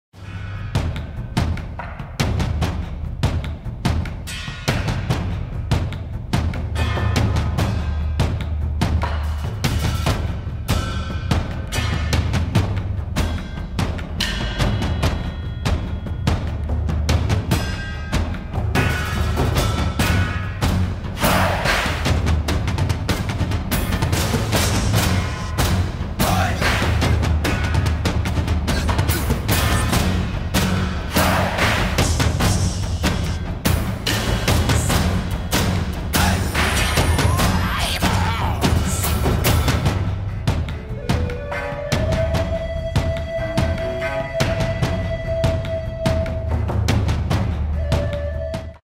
Lightsaber Duels Now Recognized as Competitive Sport in France - IGN News
The force is strong with the French Fencing Federation, which now recognizes lightsaber dueling as a competitive sport.